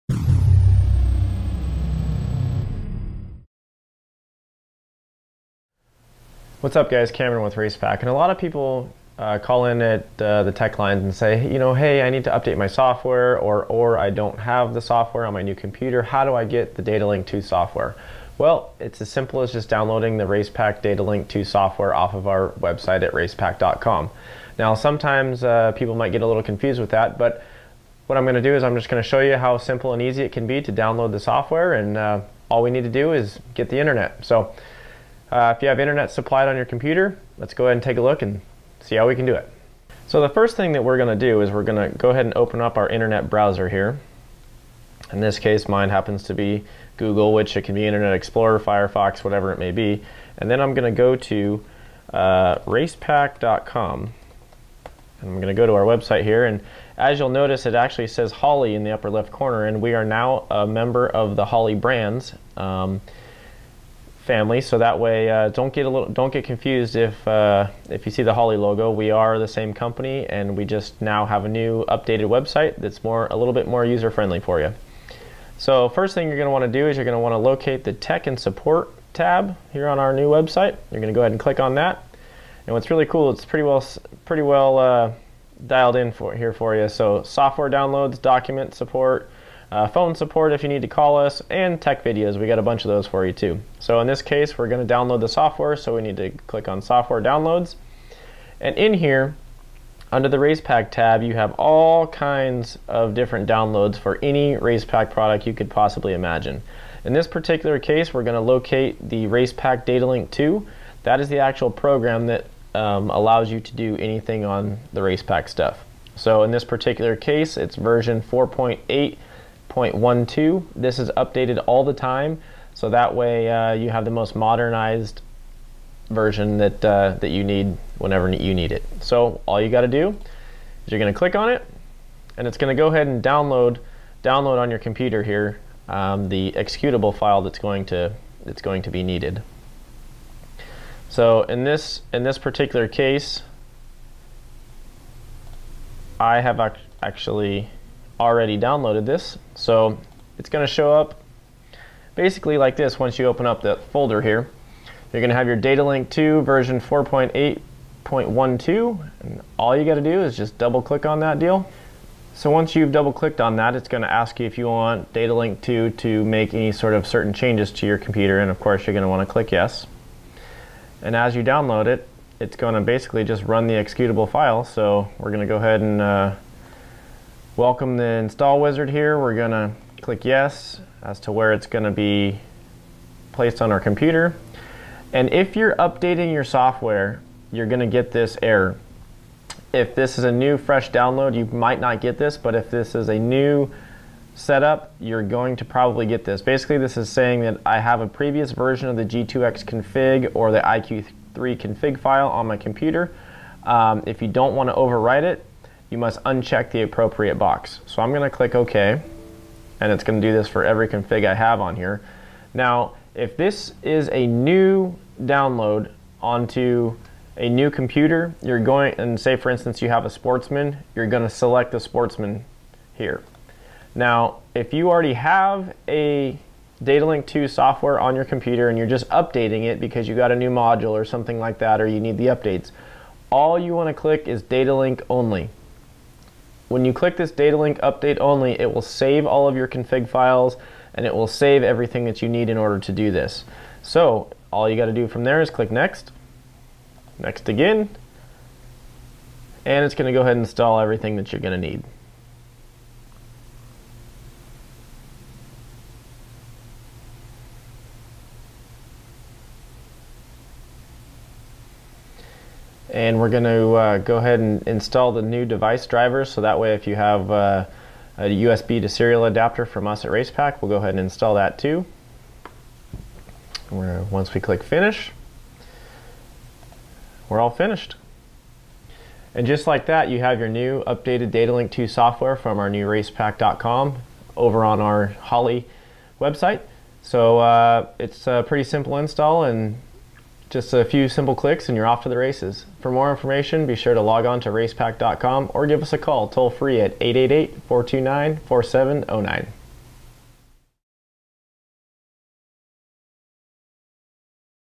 How to download Datalink II software from Racepak.com
How do you get the Racepak Datalink II software onto your computer?  Cameron walks you through how to execute downloading our software and navigating our new website.